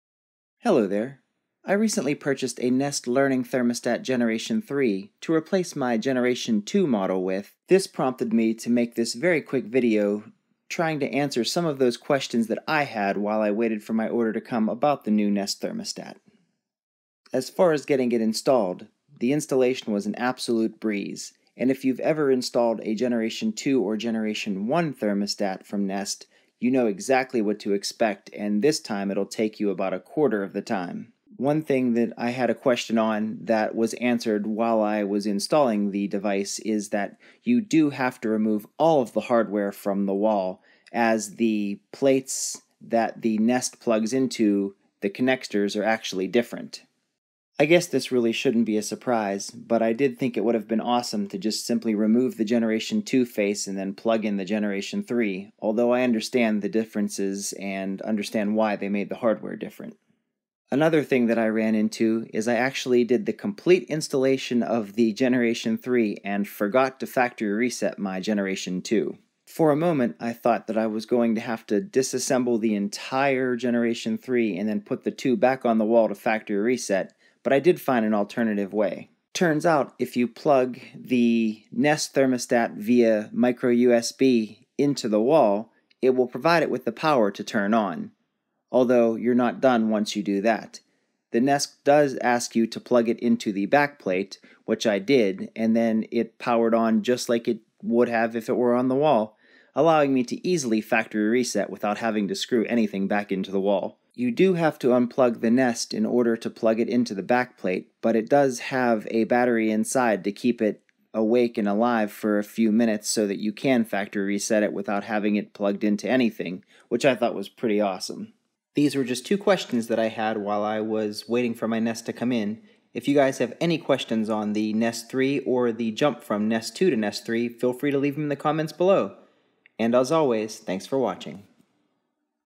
Nest Thermostat Gen 3 Questions and Answers
Answering two questions I had with the Nest Thermostat.
Do you have to remove all of the old hardware if I already own a previous nest model?  and
What if I forget to factory reset my device?